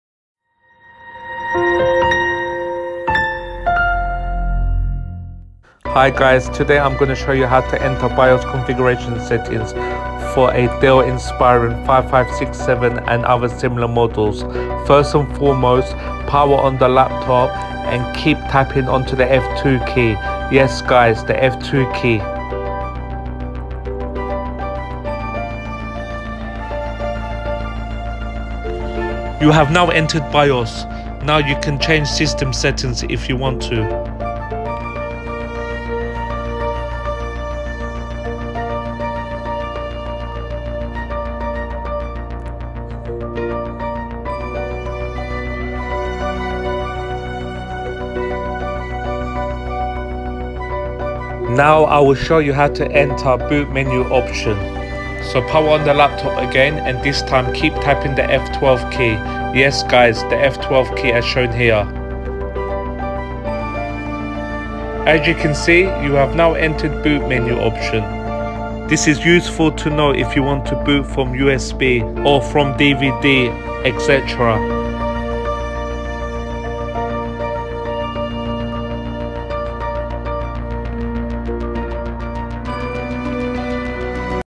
Dell Inspiron 15 5000 Series (5567) - How To Enter Bios & Boot Menu Option (Boot from USB / DVD)
Hello and welcome to FIXtastic contents.

In this tutorial, you will learn how to enter bios configuration settings if you ever want to change system settings.
Also, I will show you how to enter boot menu option. This is useful to know if you ever want to boot from usb or dvd rom

If you are new to the channel and found this video beneficial.